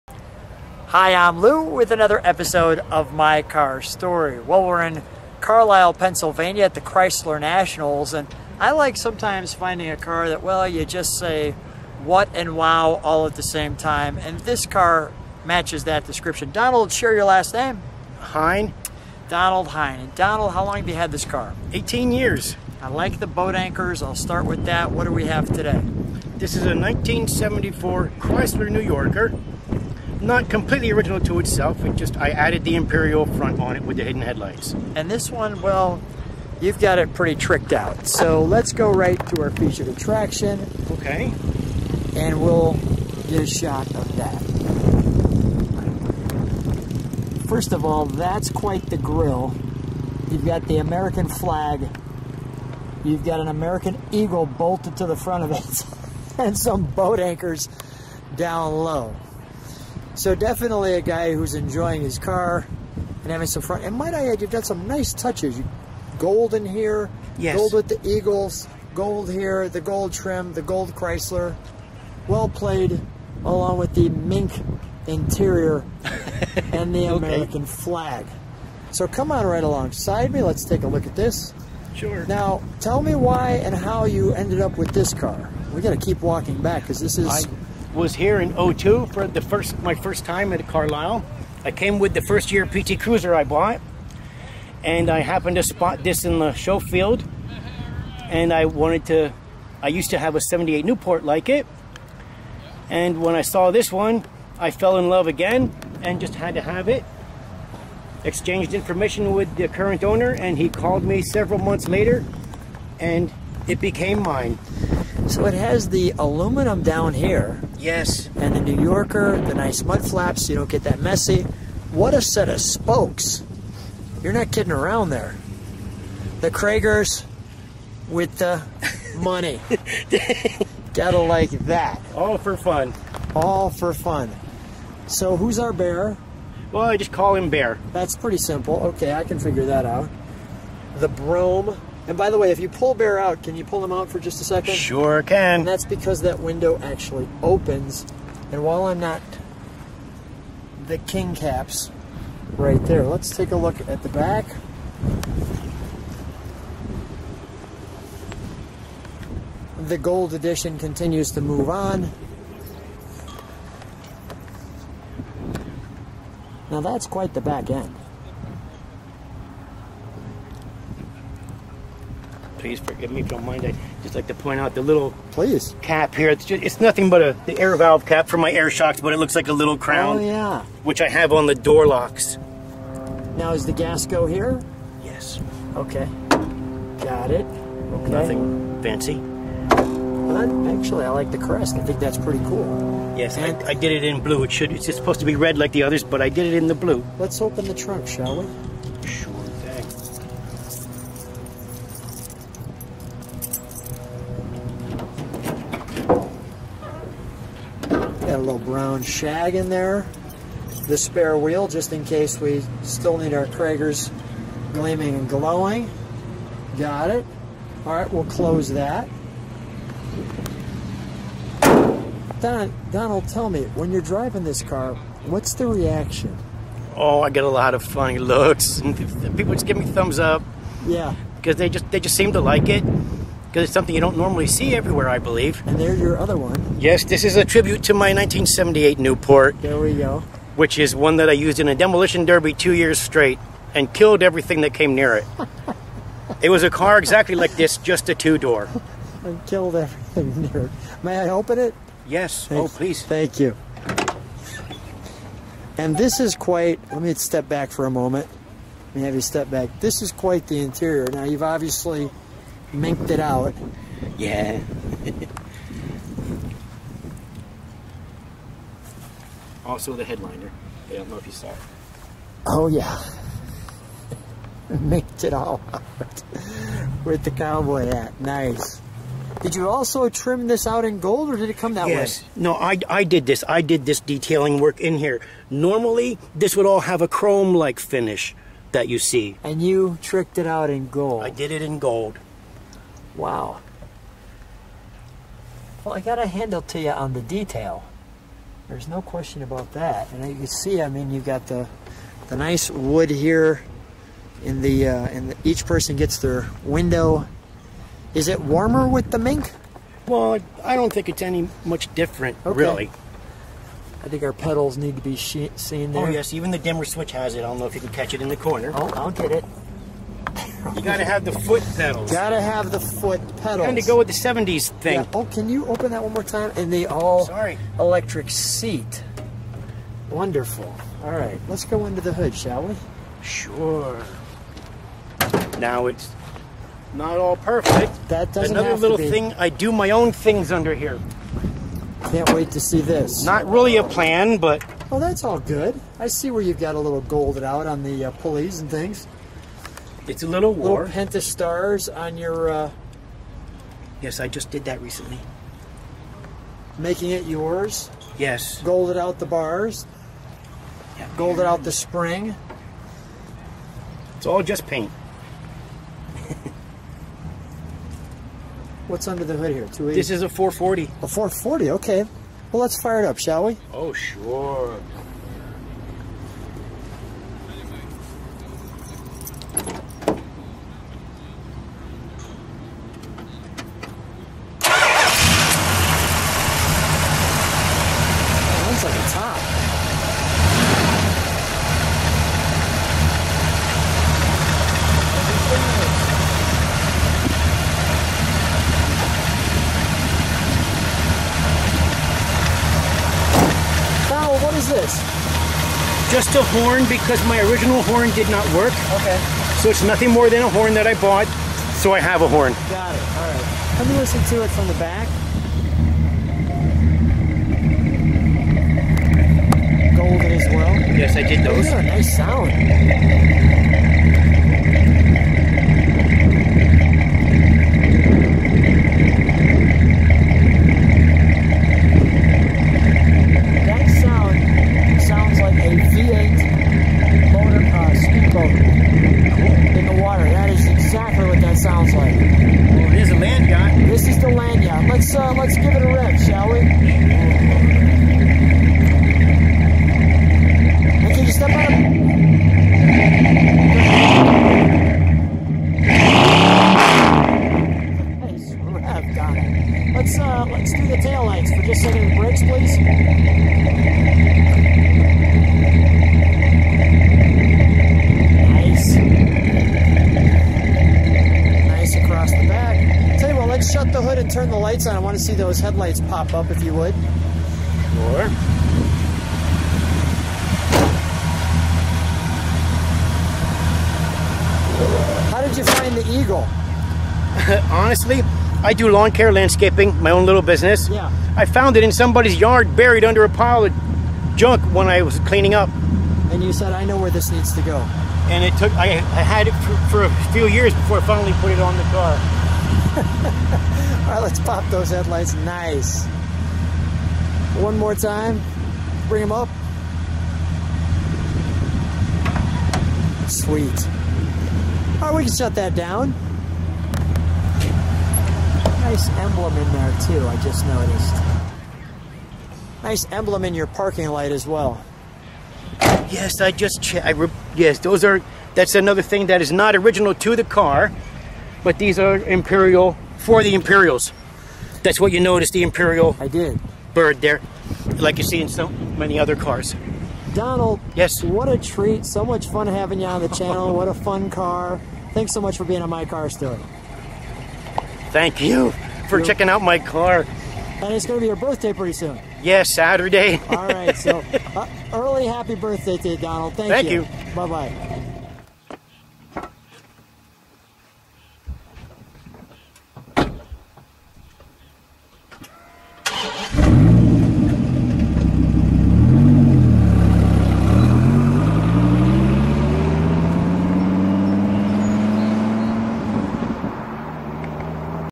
1974 Chrysler New Yorker Brougham This one is my “ Sugar Daddy “ on My Car Story with Lou Costabile
On "My Car Story" we're at the Carlisle Events in Carlisle PA on 7-9-20.

We're looking at a 1974 Chrysler New Yorker Brougham in White Paint.

The car's Owner is Donald Hein.  Donald’s had this car since 2002.  He shares he ran to this car when he seen it and was amazed with the cars looks.  He shared his phone number with the Owner, and a few months later the Owner shared he could buy it and did.  He enjoys customizing this car with his own special touches and has named his car “ Crystal”.

If you’re interested in learning about the different events at the Carlisle Events, go to the